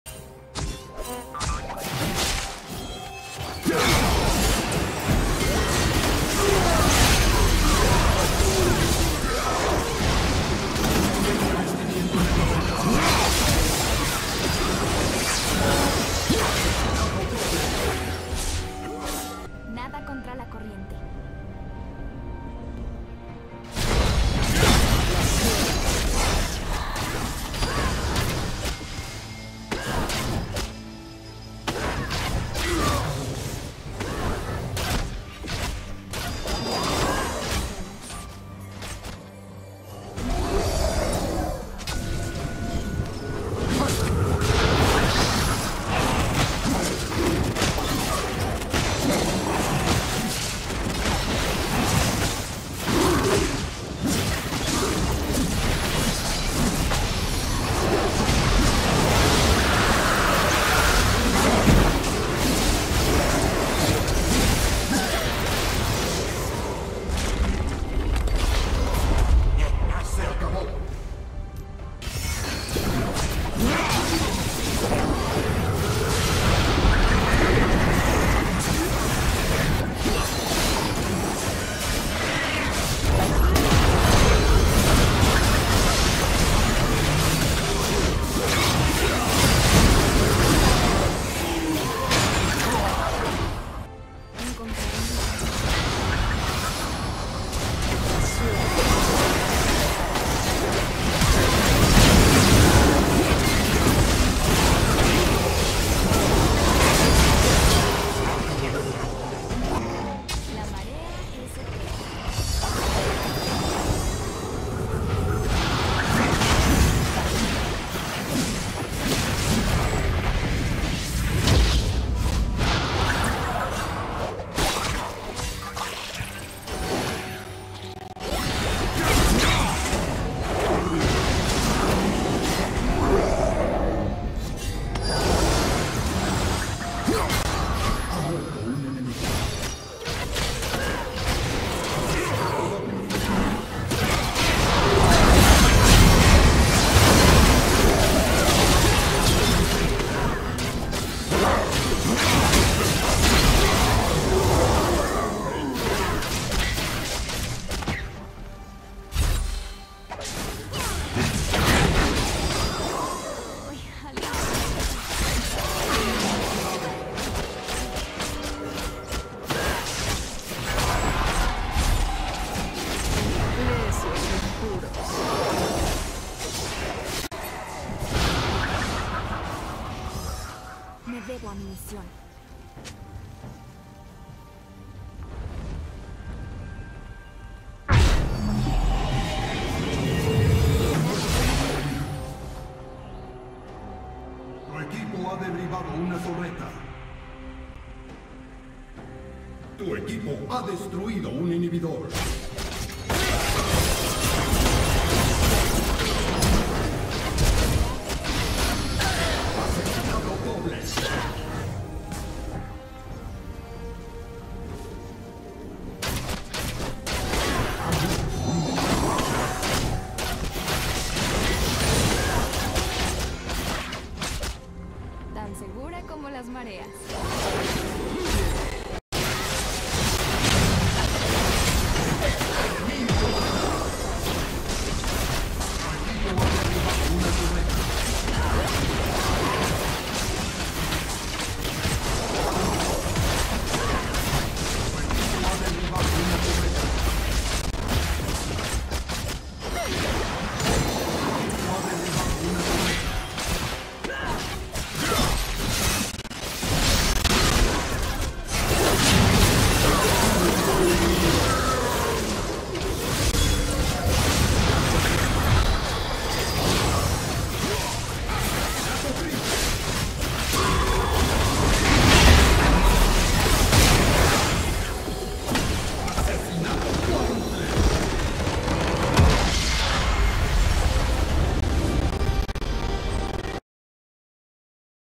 NamixVayne
Antes de nada... ¡FELIZ AÑO NUEVO 2O2O!
Gameplay de Lord y Lady Spidey en duo *.* Primer vídeo del 2020
Visiten nuestra página de Facebook, tenemos sorteo solo para LAN